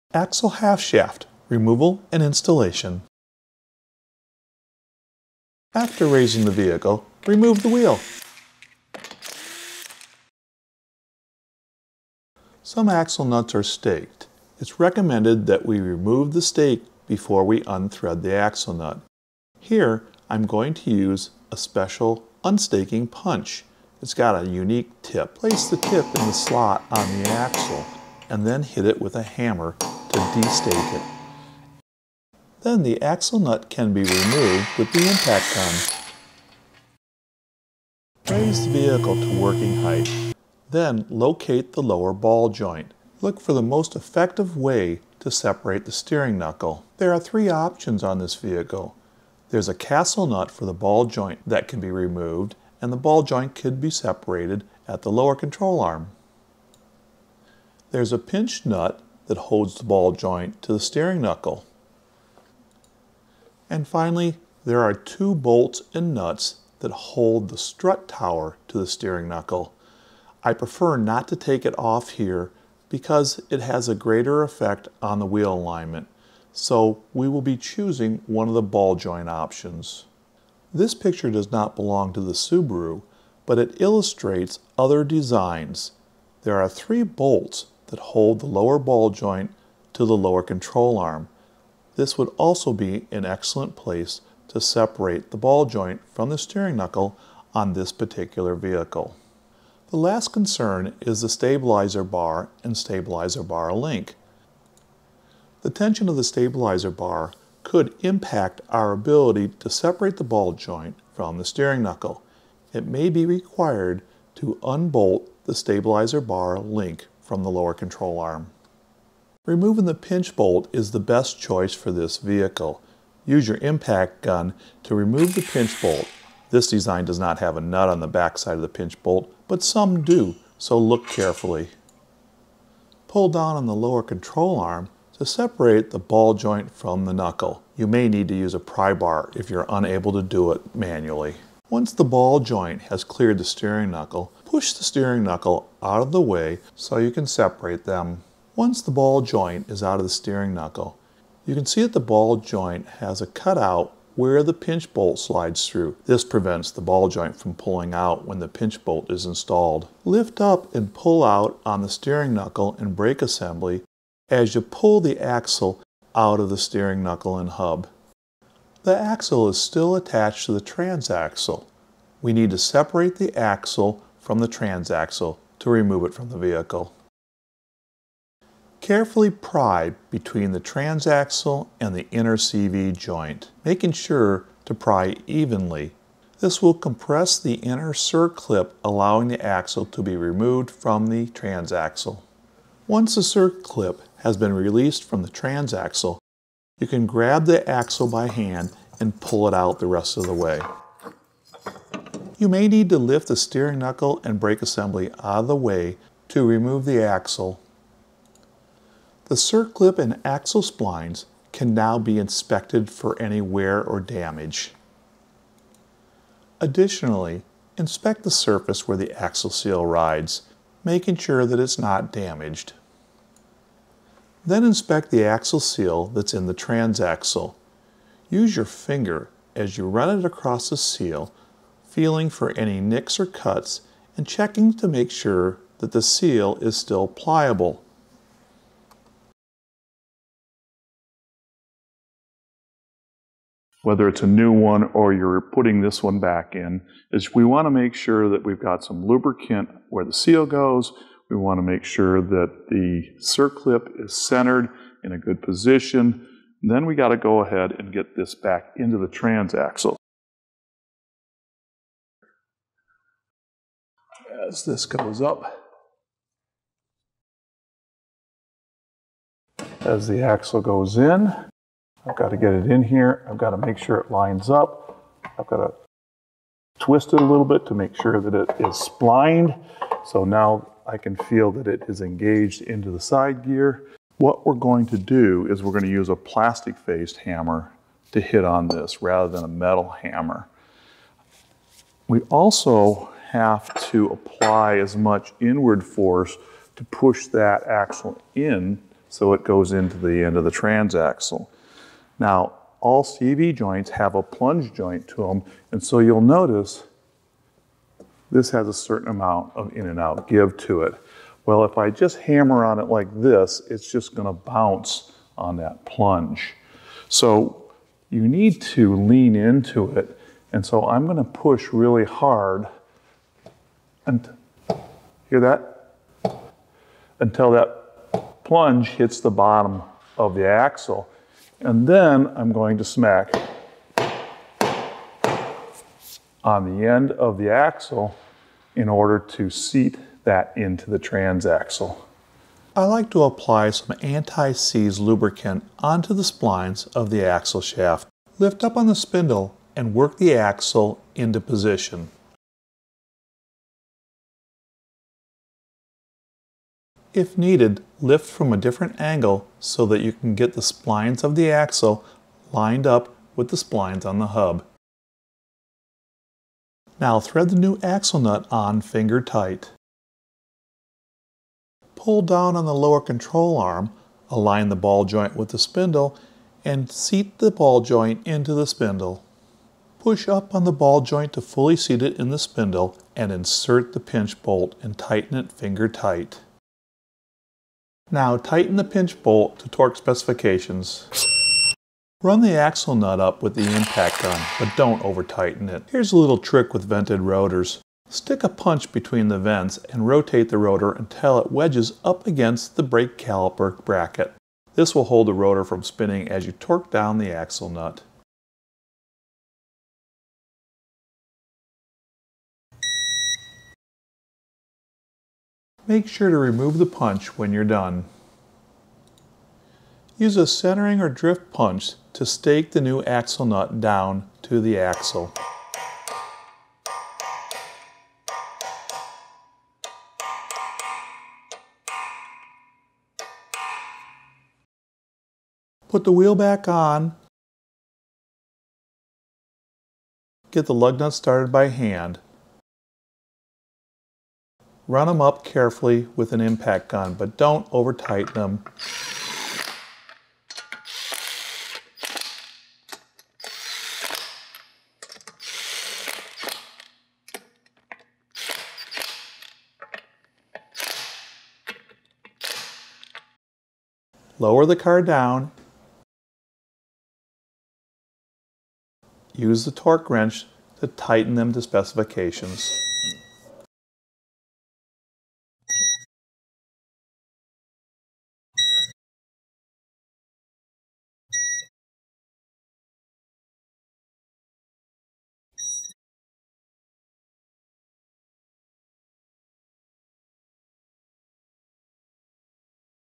Axle Half Shaft Removal & Installation
Educational demonstration on how to remove and install a axle half shaft also known as a CV axle on a 2015 Subaru WRX.